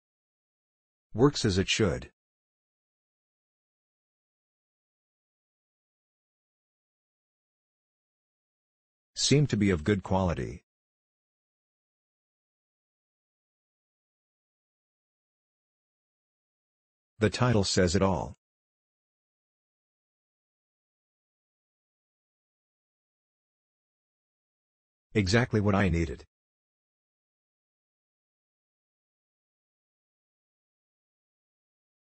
bROK Products 15939 1-7/8"x5/8" x1-3/4 Lawn Mower Hitch Ball - 2000 lb.
Works as it should Seemed to be of good quality. The title says it all Exactly what I needed

Enables one wrench to tighten on bROK and hex recess ball mounts
Oversized platform base for improved support over traditional hitch balls
Backwards and forward integration with bROK, hex, standard and heavy-duty forged ball mounts
1-7/8 inch diameter, 5/8 inch shank diameter, 1-3/4 inch shank length
2,000 lb. capacity